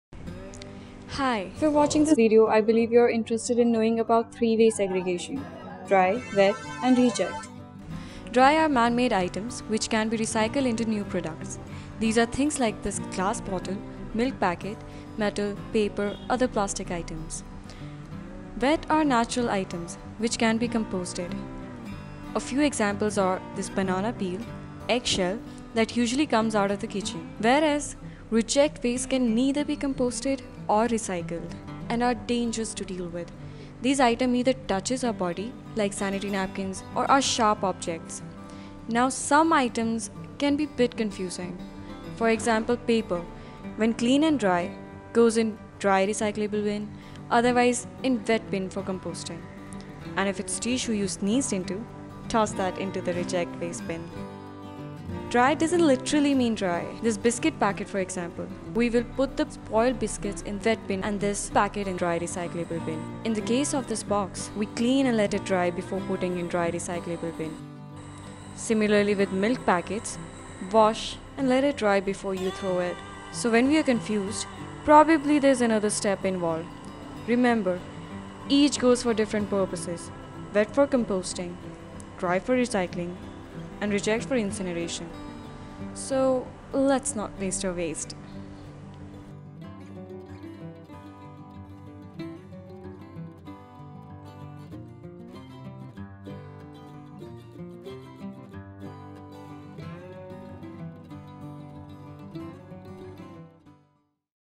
3 way segregation of waste - English
Anything that has be discarded has to be segregated so that it can be used, otherwise it all ends up in landfills. It has to be at least these 3 categories to retain items' usability.